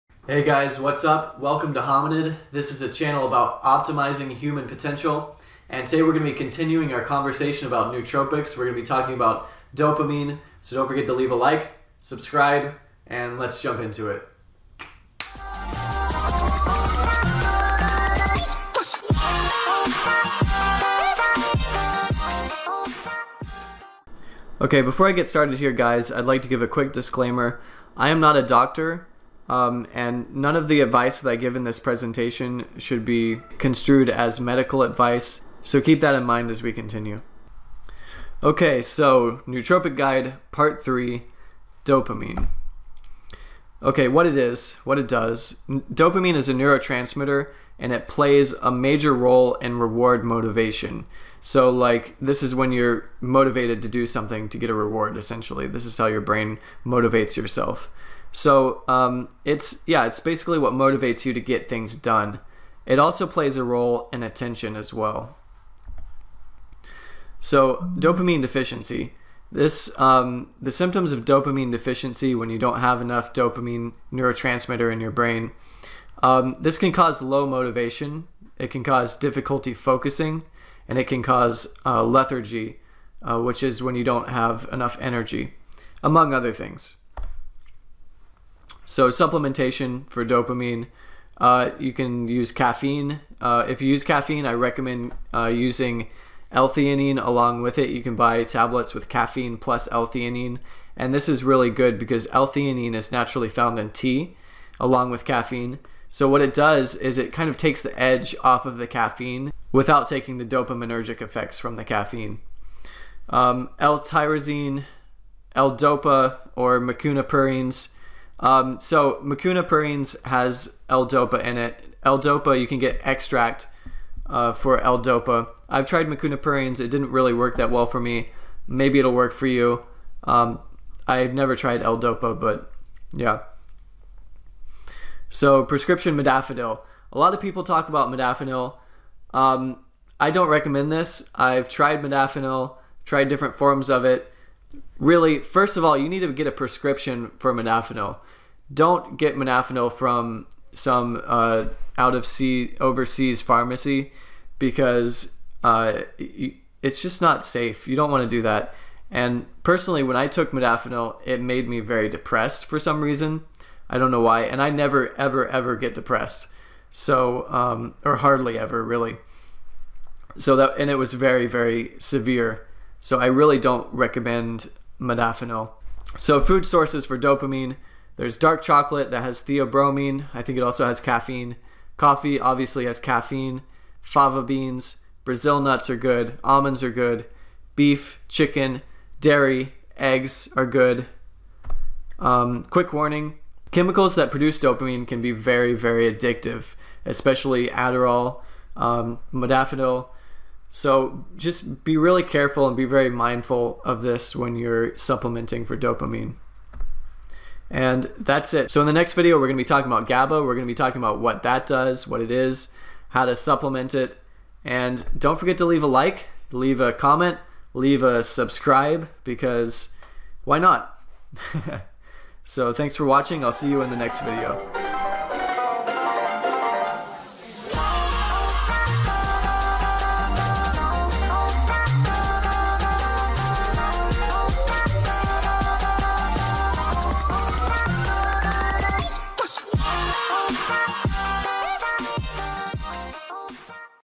Nootropics Guide Pt. 3 | Dopamine
In this video guide I talk all about nootropics and how you properly supplement dopamine for optimal nootropic supplementation. Dopamine is a neurotransmitter than is vital for motivation. It's important to analyze your deficits when you supplement with nootropics, because it provides you with a guide for what nootropics you should be taking. This video is Pt. 3 of a 5 Pt. guide.